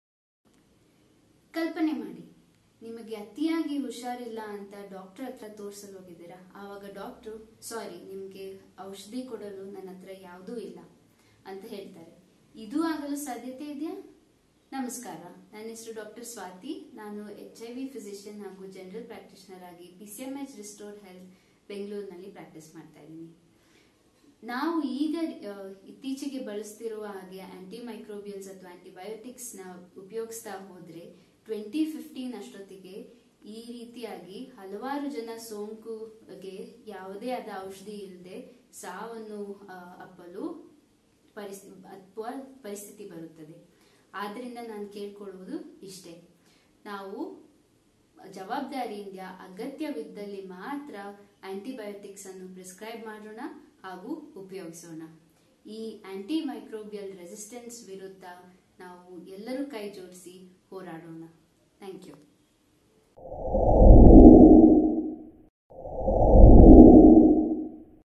World Antimicrobial Awareness Week : Message by Dr. Swathi balachandra, Lead Spice Route Karnataka
World Antimicrobial Awareness Week (WAAW) aims to increase awareness of global antimicrobial resistance and to encourage best practices among the general public, health workers and policy makers to avoid the further emergence and spread of drug-resistant infections.
Message by Dr. Swathi balachandra, Lead Spice Route Karnataka